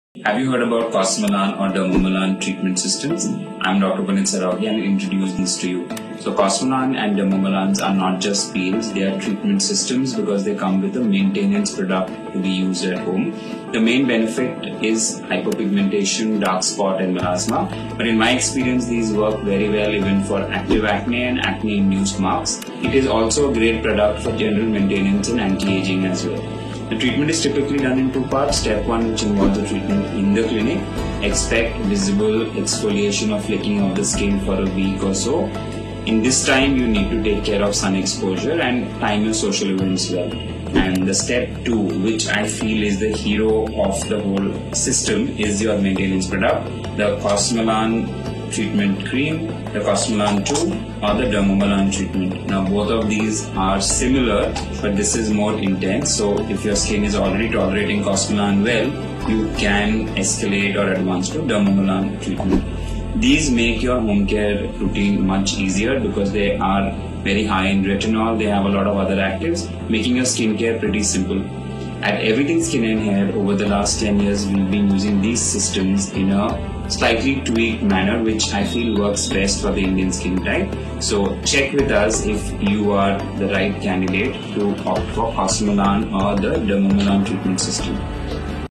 Cosmelan & Dermamalen Treatments | Anti - Aging | Boosts Collagen | Treats Spots & Pigmentation
The cosmelan & Dermamelan treatment systems impressively tackles pigmentation by effectively treating spots while regulating excessive pigmentation, ensuring both immediate improvement and long-term prevention✨
Its added benefit of boosting collagen and elastin production aids in anti-aging, promoting healthier and more resilient skin🤩
To optimize results for Indian skin types, we've fine-tuned the protocol, tailoring it to better suit the needs and sensitivities commonly observed in Indian skin🙌🏻
Always seek guidance from your dermatologist for personalized treatment recommendations👍🏻

📝Procedure - step 1 - In clinic treatment and step 2 - Home Care
🤗Best for - Pigmentation, Acne, Antiageing, dull skin
⏰Duration of the Treatment - 30 mins
🗓️Downtime - 1 week to 10 days, where the skin is peeling out and needs protection from sun
😨Pain - Nil
☑️Results - last upto 6-12 months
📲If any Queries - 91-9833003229
📍Location - Juhu, Andheri, Mumbai